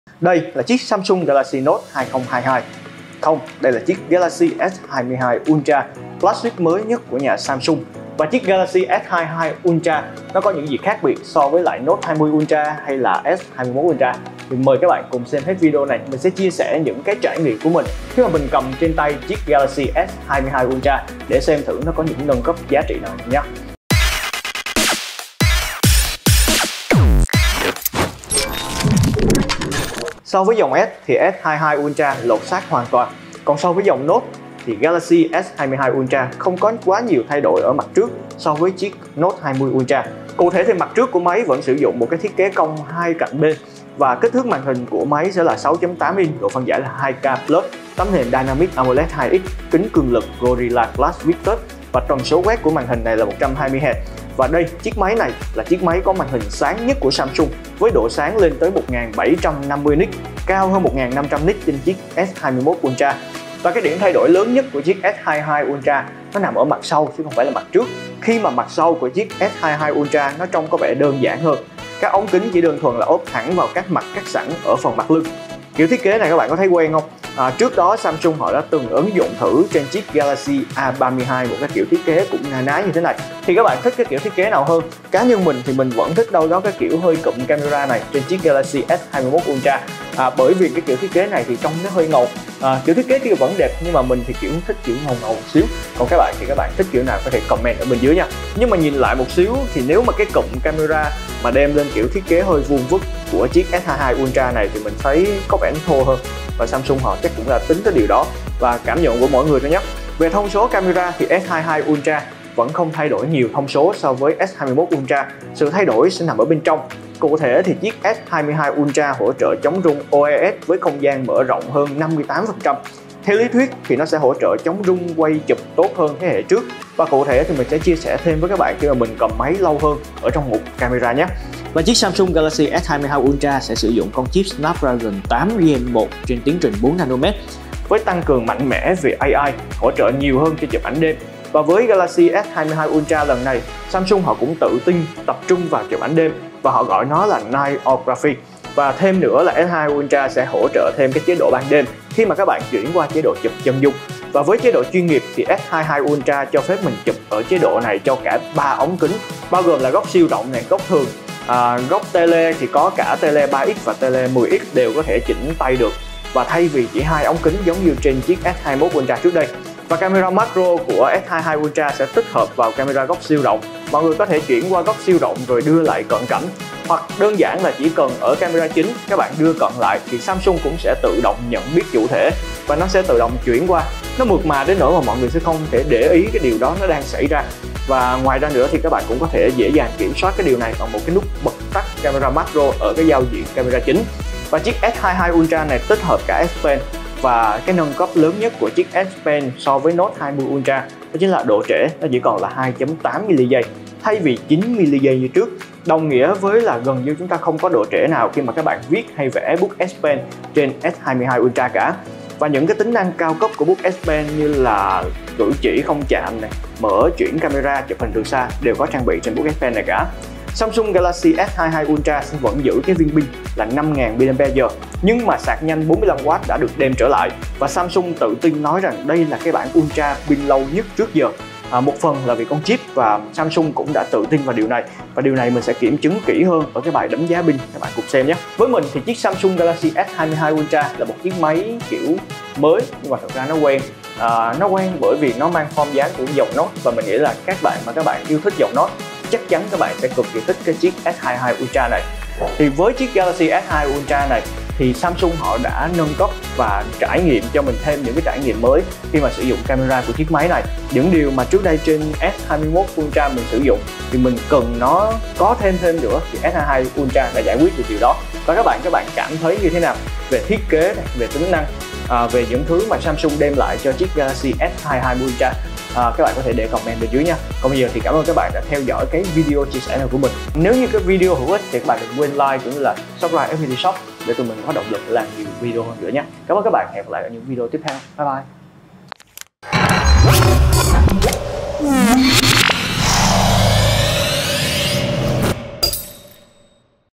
Trải nghiệm những thay đổi trên Samsung Galaxy S22 Ultra
Samsung Galaxy S22 Ultra có vẻ như không có quá nhiều thay đổi? Không đâu, có nhiều điểm nâng cấp trên chiếc máy này. Các bạn cùng xem video nhé!

00:00 Giới thiệu
00:30 Thiết kế
01:53 Camera
03:15 Bút Spen
04:20 Trải nghiệm sử dụng